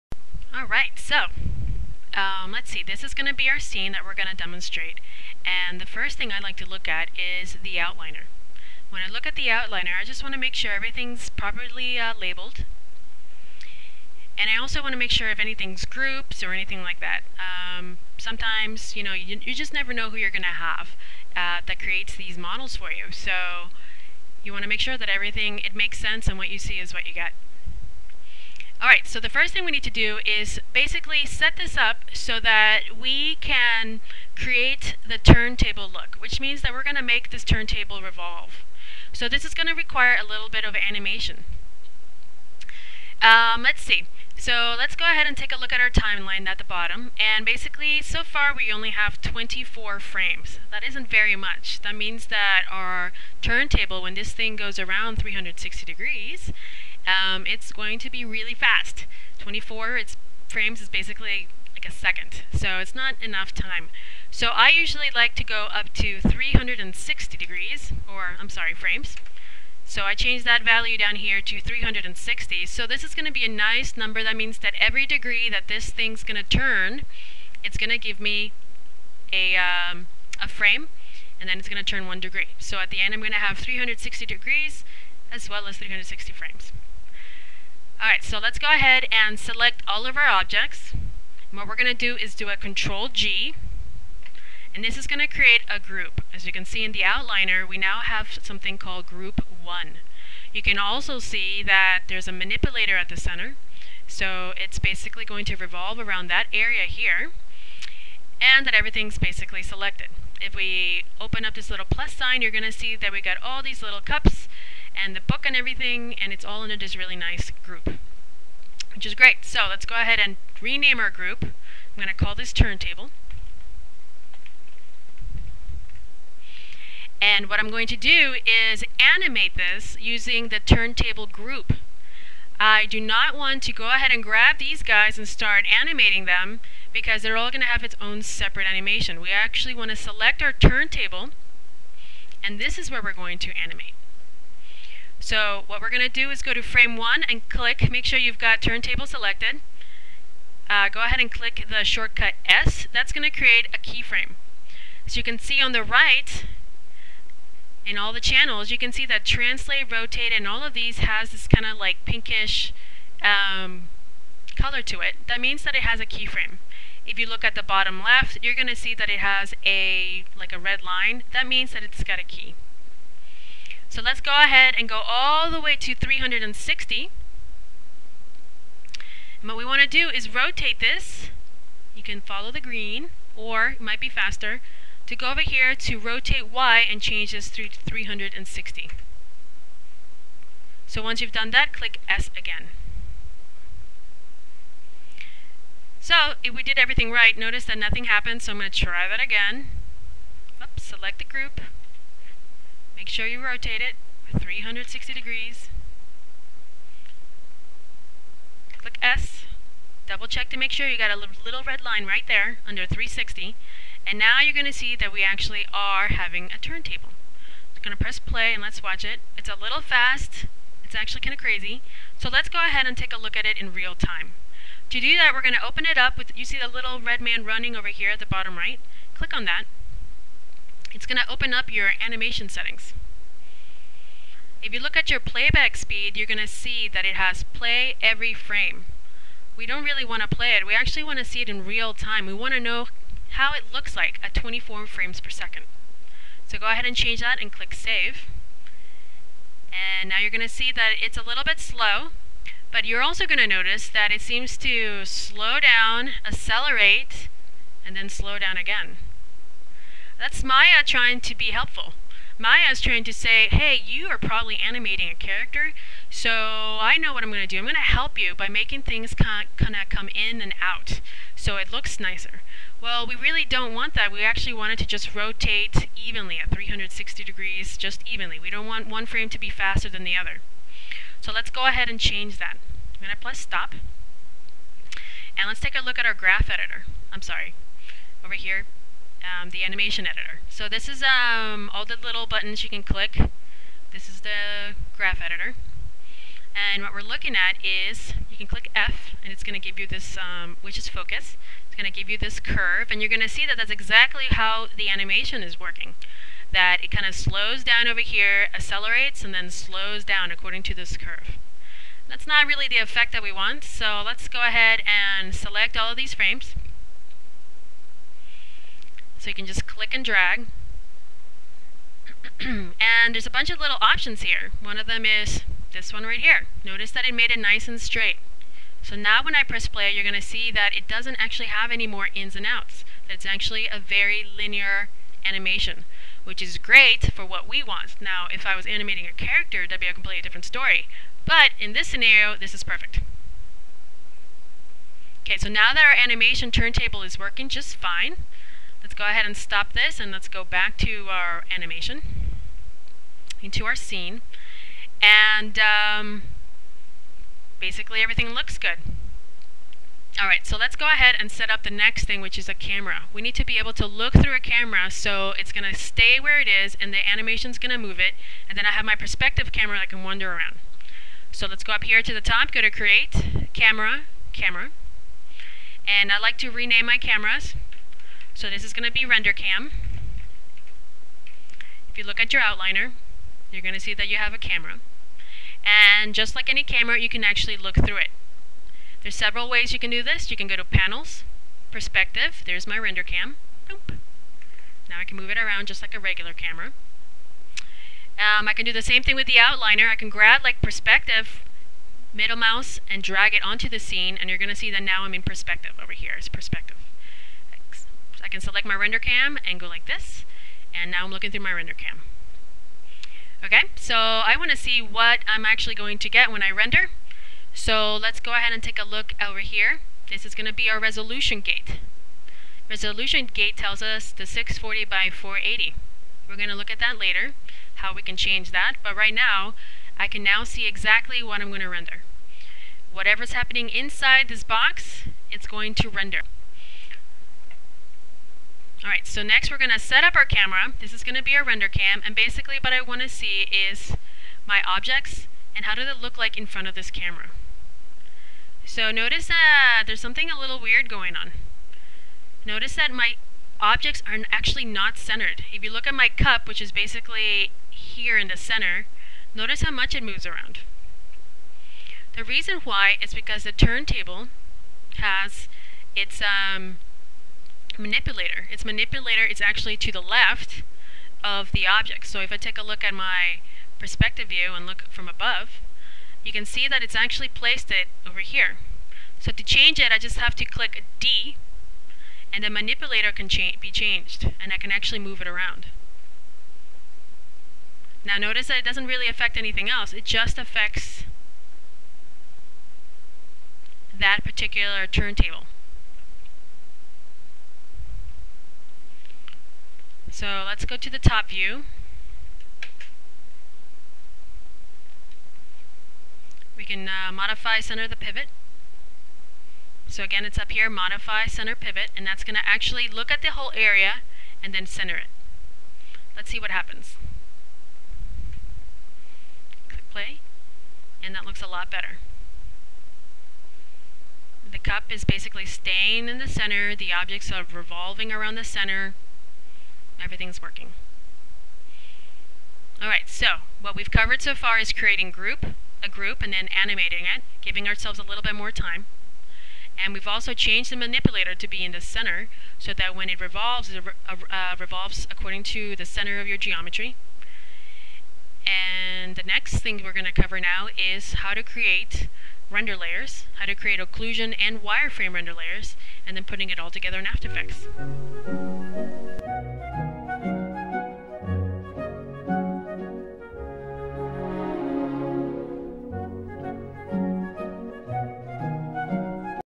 Maya 102: How to Create a Wireframe Occlusion Turntable (1/4)
This video covers how to create render layers to produce Occlusion and Wireframe.